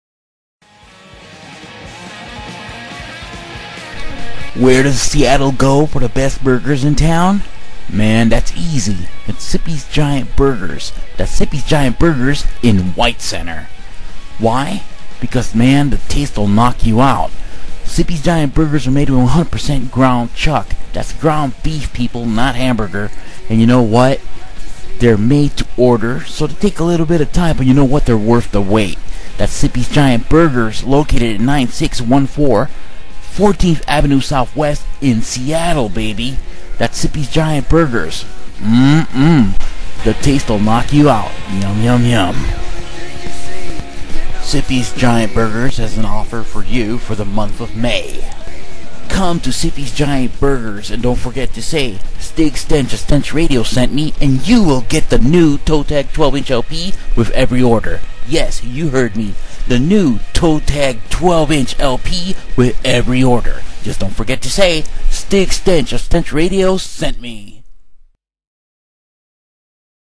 Zippy's Giant Burgers (White Center) + SPECIAL May Free Toe Tag 12" LP Offer!
You want to go to eat at a place that rocks?
Come to SEATTLE's one-and-only ZIPPY's Giant Burgers! 1/3-pound patties make for big burgers at this fast-food spot with vintage sodas & retro digs.

Located in White Center at
9614 14th Avenue SW, Seattle WA 98106!

Business Hours:

M-Th        8a.m.-9p.m.
F-S.          8a.m.-10p.m.
Sun.         9a.m.-9p.m.

The Taste Will Knock You Out!
Yum!
Yum!!
YUM!!!